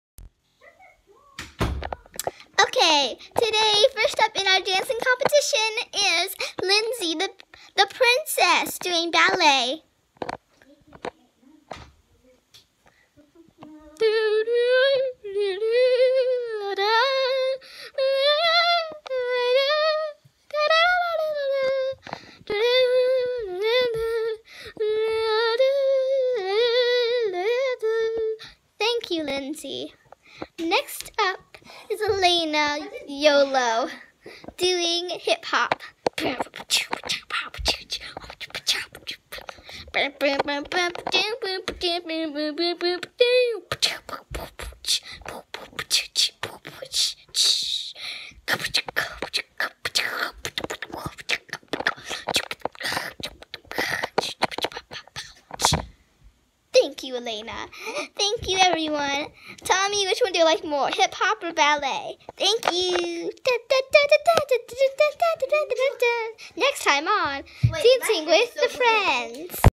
Dancing with the friends,a play
This is a play about hip hop and ballet,tell us which one you like more in the comments! Enjoy!!!